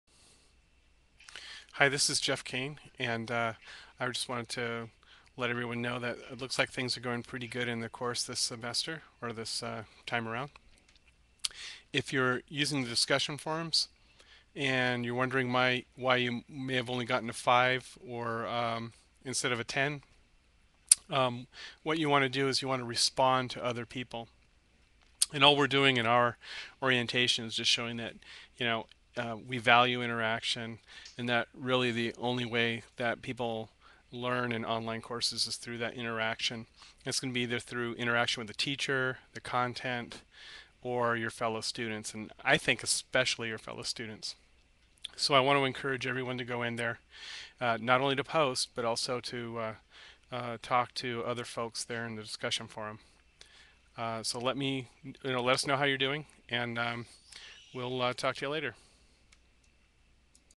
Interaction in online courses...
A quick note to students about the importance of interactivity in our online courses.